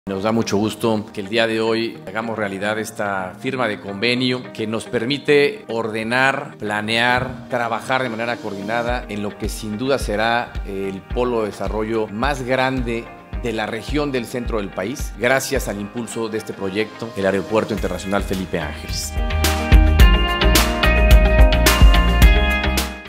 Firma de convenio con el Gobierno de Hidalgo y el Gobierno de México.
El convenio que firmamos con el Gobierno de Hidalgo y el Gobierno de México, permitirá que los tres órdenes de gobierno trabajemos de manera coordinada y ordenada en el Nuevo Aeropuerto Internacional “Felipe Ángeles”.‬